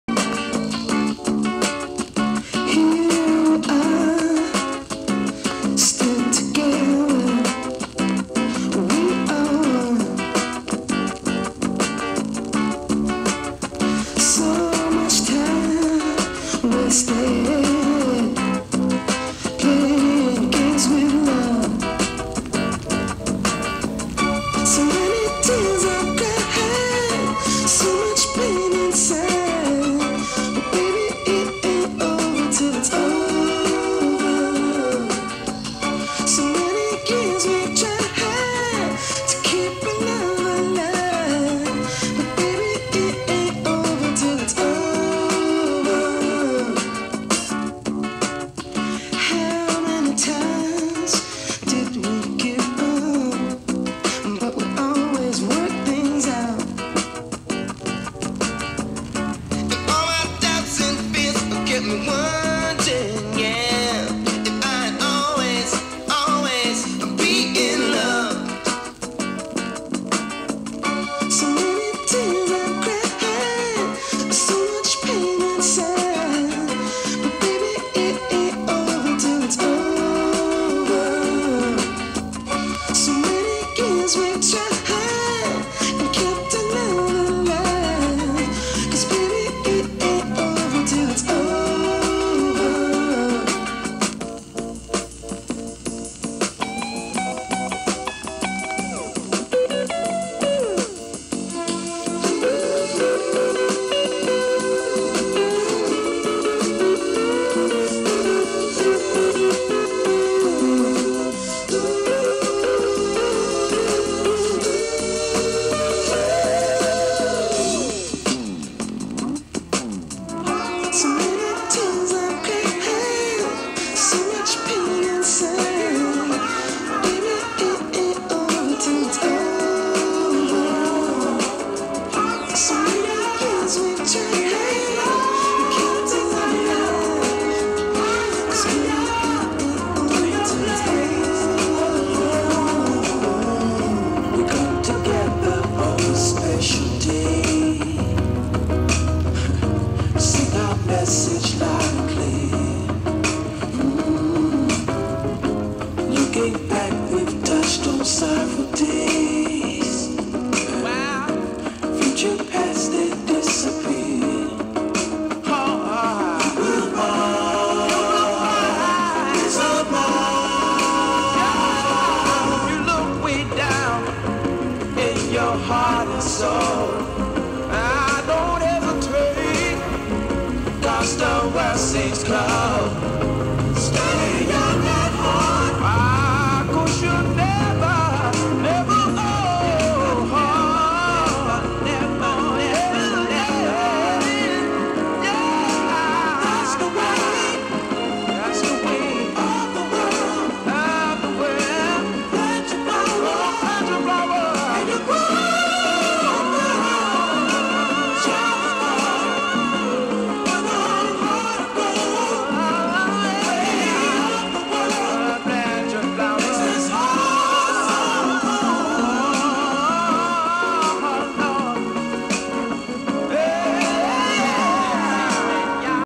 Off the Grid Smooth Blends - All 45s Vinyl Mix
DJ anywhere with my battery powered portable DJ rig... Check it out!